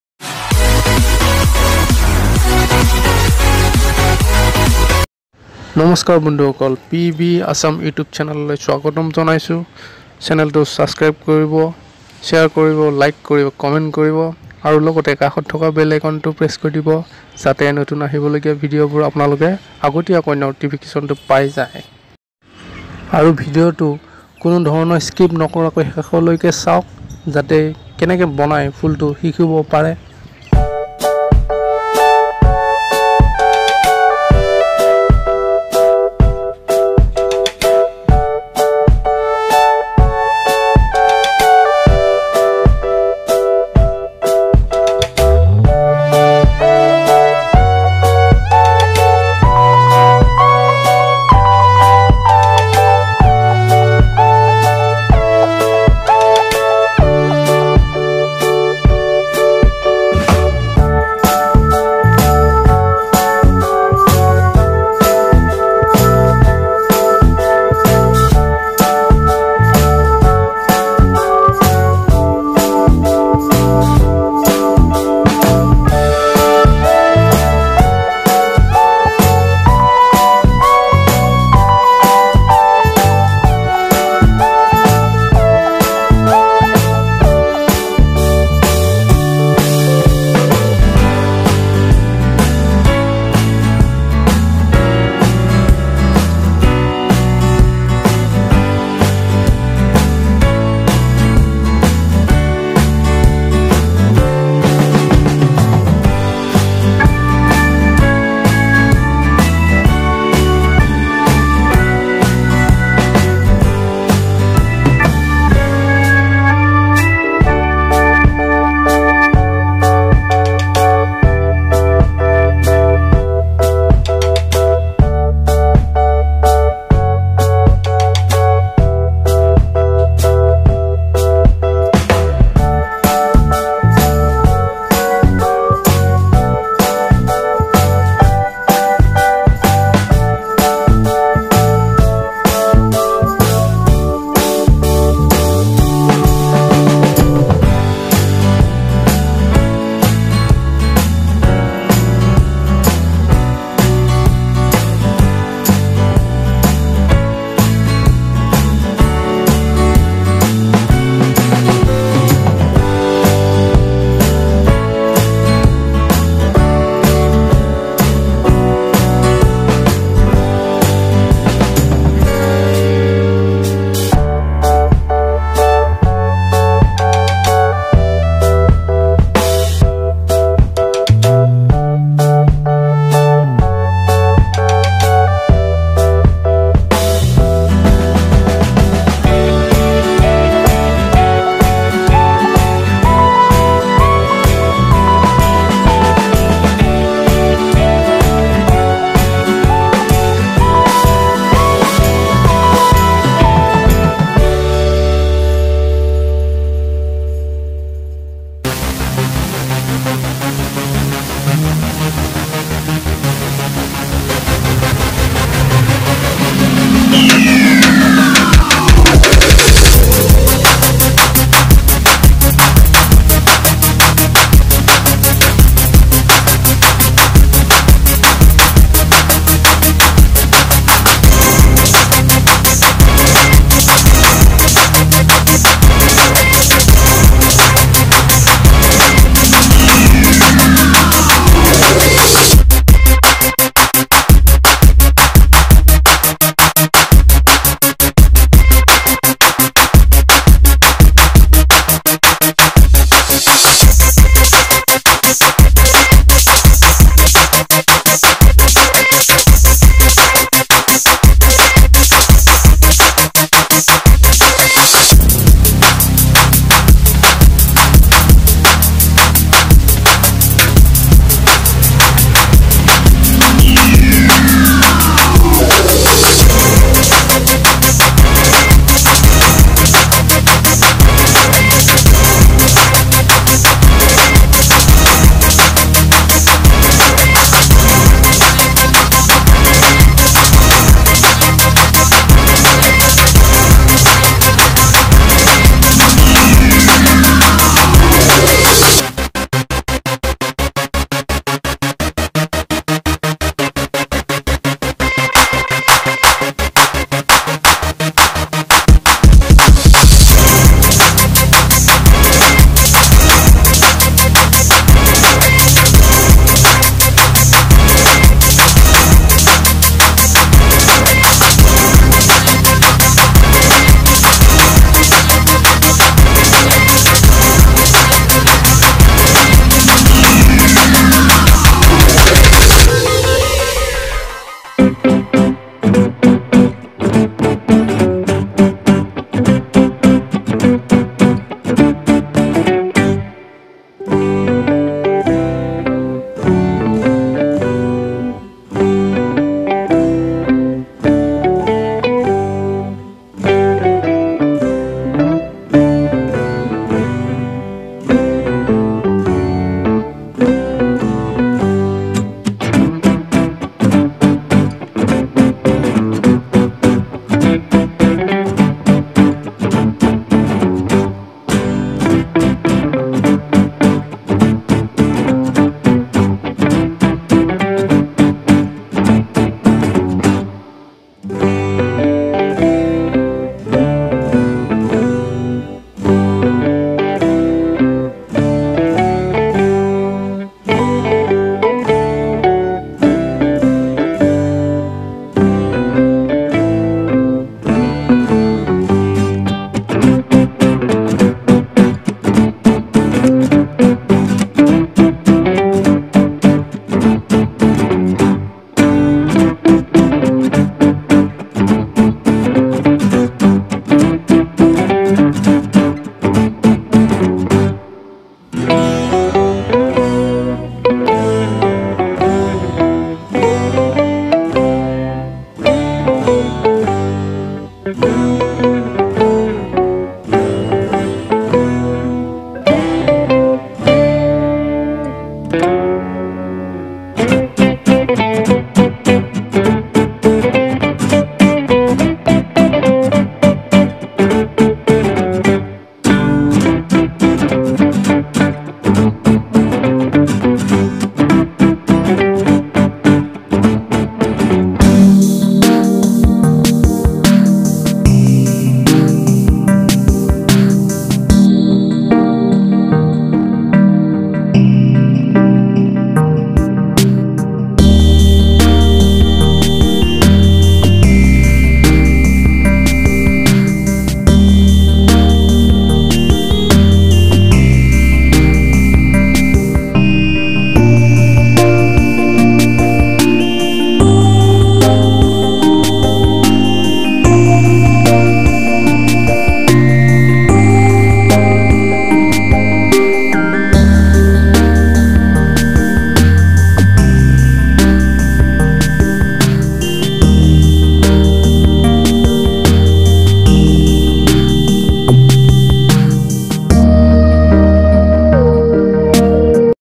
Diy Paper n woolen wall hanging flower
উল আৰু কাগজৰ ঘৰৰ বেৰাত সজাবলৈ নতুন এটা মদেলৰ ফুল সাজিলো ৷ মই বনোৱাৰ দৰে সাজিব পাৰিব সকলোৱে ইমান টান নহয় মোৰ ভিডিও তো চালে নিজে বনাব পাৰিব ৷ আৰু যদি কাৰোবাক ফুল নিজৰ ঘৰৰ কাৰনে লাগে মোৰ কাখত দিয়া নম্বৰ টোত ফোন কৰি যোগাযোগ কৰিব পাৰিব ৷